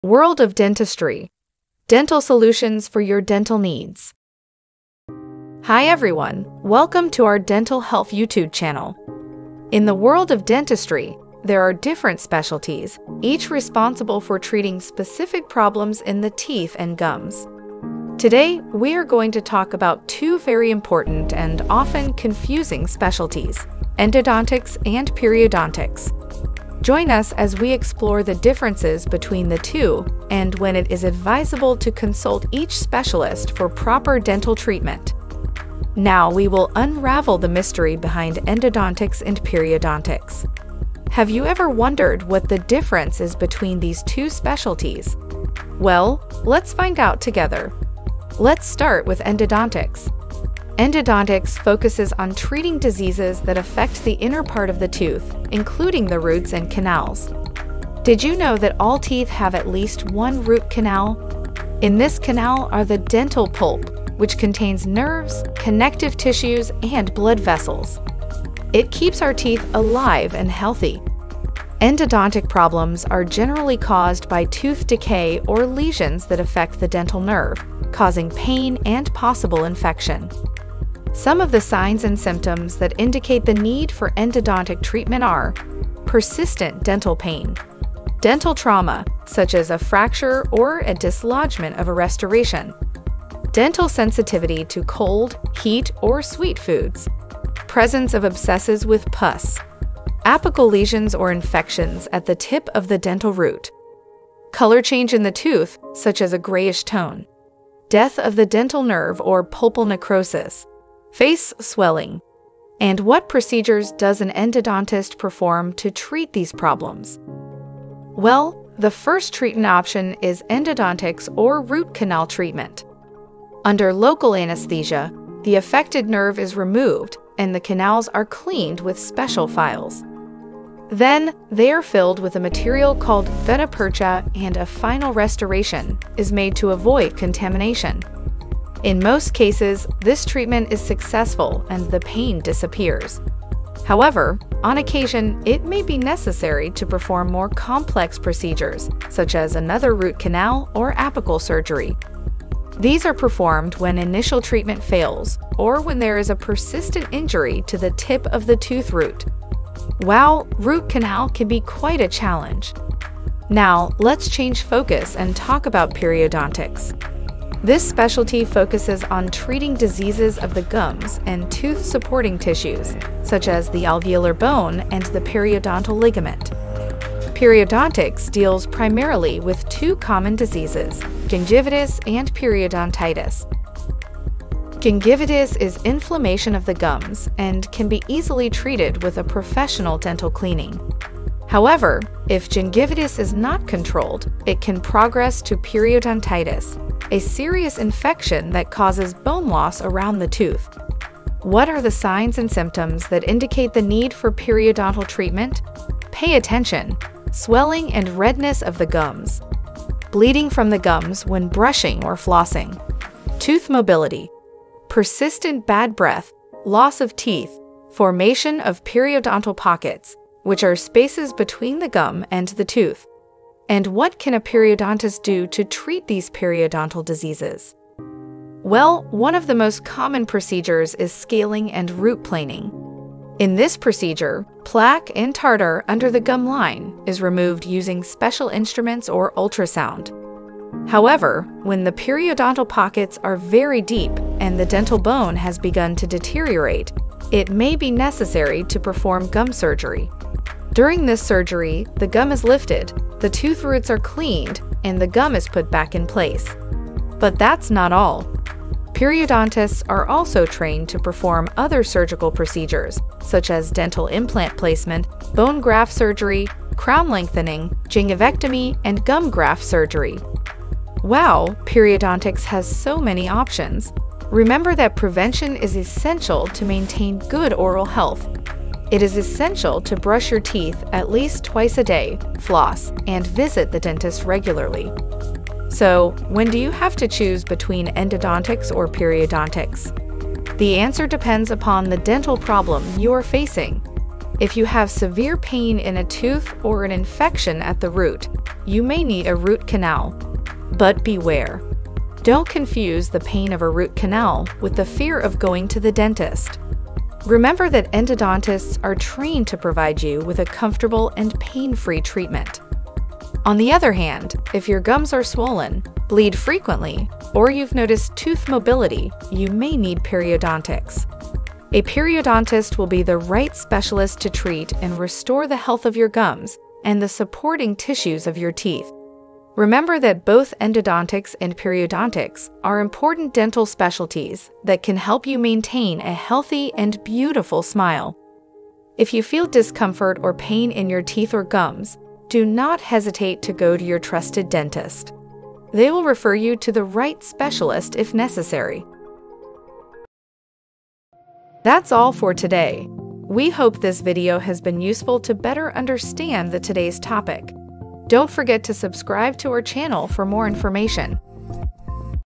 Endodontist vs Periodontist: 3 Surgeries That Differ Them
Confused about whether to consult an endodontist or a periodontist for your dental concerns? This video breaks down the differences between these two dental specialties and guides you on when it's best to seek treatment from each specialist. Dentistry is a vast field, and while general dentists can handle many procedures, complex cases often require specialized care. Discover the role of an endodontist, who focuses on diseases of the tooth's inner part, such as root canals, dental pulp, and associated pain. Learn about the signs and symptoms that indicate the need for endodontic treatment. Explore the various procedures performed by endodontists, including root canal treatment, retreatment, and apical surgery. On the other hand, delve into the realm of periodontics, where periodontists specialize in gum and tooth-supporting tissue diseases. Uncover the signs and symptoms that necessitate periodontal treatment, such as gum swelling, bleeding, and tooth mobility. Gain insights into the treatment options offered by periodontists, such as scaling and root planing, gum surgery, dental implant placement, and more. Discover the importance of prevention through good oral hygiene practices and regular dental check-ups. While general dentists can handle certain cases, more complex situations may require the expertise of an endodontist or periodontist. Don't let dental concerns linger—watch this video to understand the differences between endodontics and periodontics and make informed decisions about your dental health.